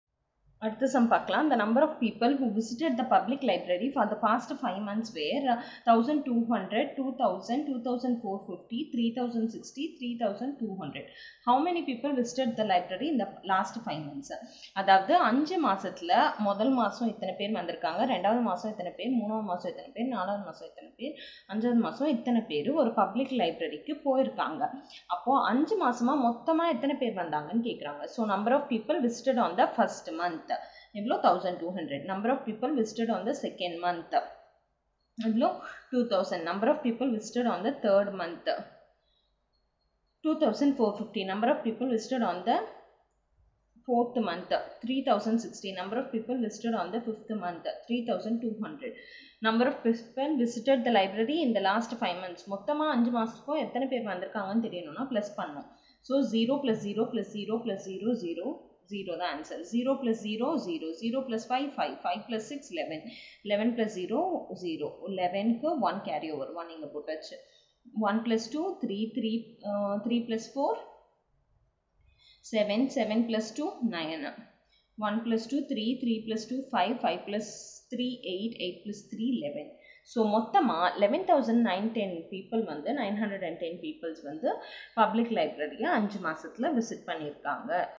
Samacheer kalvi/6th std/mathematics/chap-1/numbers/exercise 1.3
3. The number of people who visited the public library for the past 5 months were 1200, 2000, 2450, 3060 and 3200. How many people visited the library in the last 5 months?